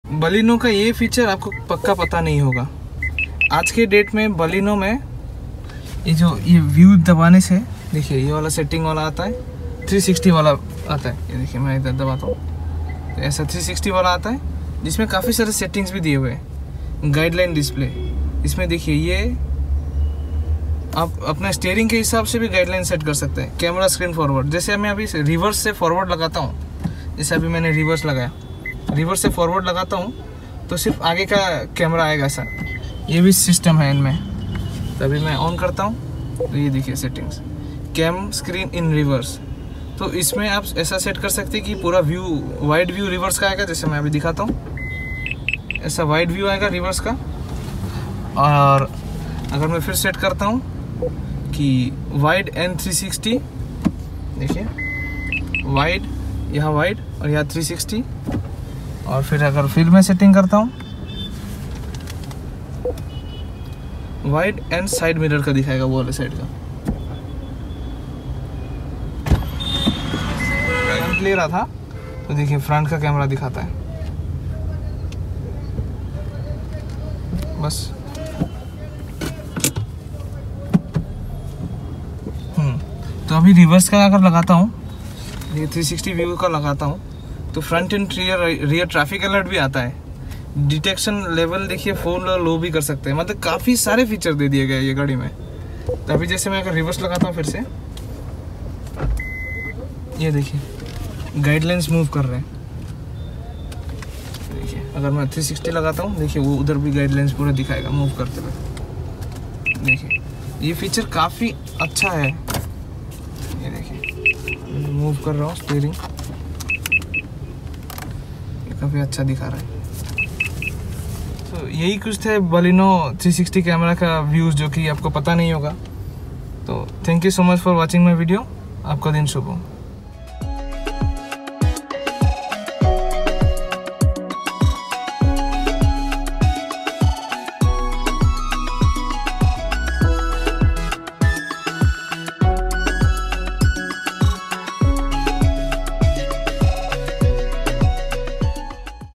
Most Detailed Baleno 360 Camera And Reverse Camera Features | Enable adaptive guidelines in Baleno
Hello And Welcome to CAR SARKAR

Today i will be showing you the Features  of Hyundai i20 2019 Asta Optional